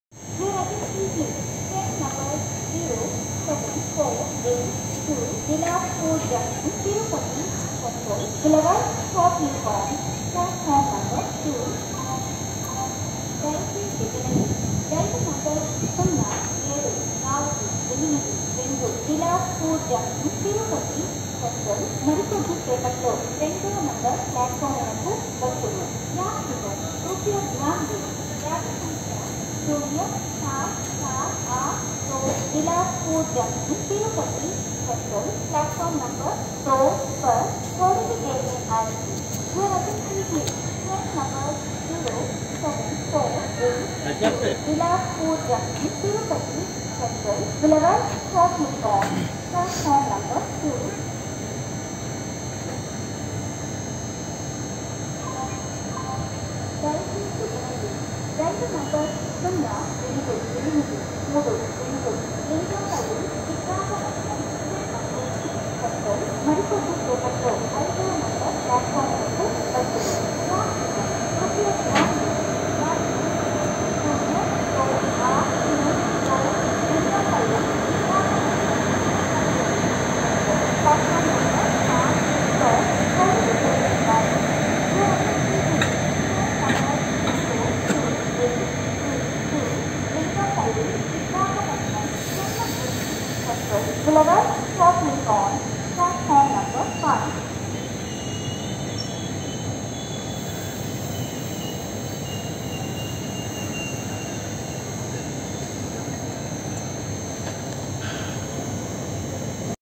TRAIN NO. 02831 VISAKHAPATNAM TO LINGAMPALLI S/F EXPRESS
Vishakapatnam Lingampalli SF Exp arriving at Tenali jn (Andra Pradesh).